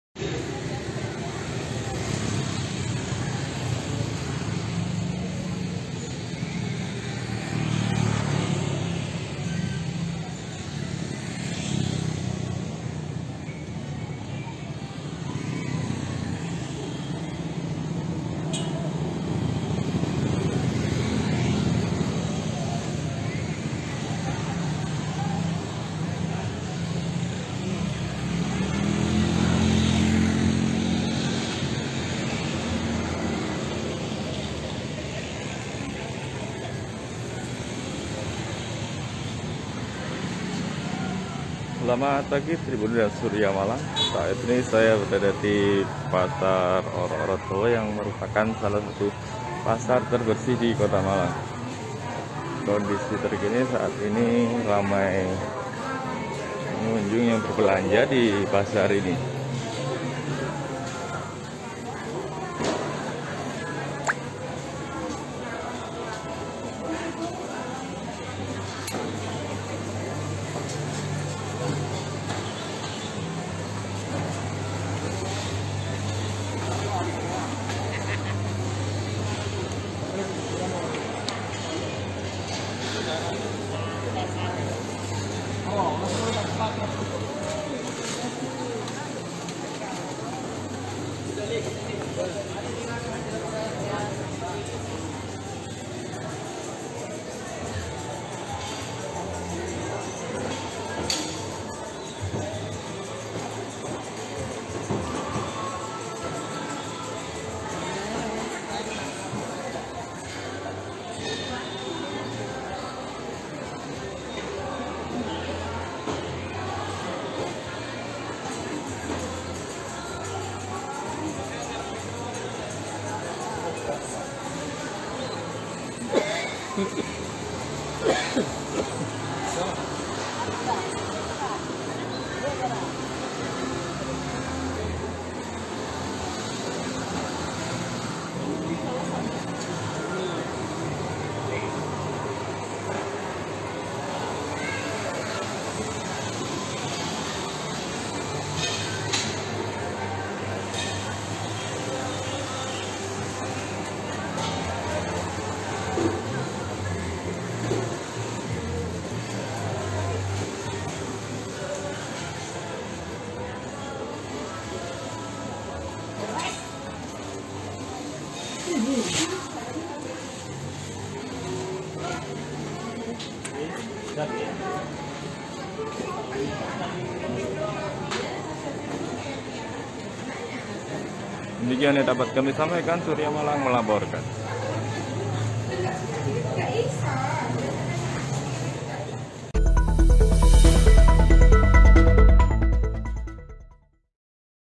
Suasana Pasar Oro-Oro Dowo Yang Merupakan Salah Satu Pasar Terbersih di Malang
Reporter : Rizal Van Nani - Editor Video : Akhmad Yusuf
MALANG - SURYAMALANG.COM

Suasana pasar oro-oro dowo yang merupakan salah satu pasar terbersih di Malang, Senin 22 Agustus 2022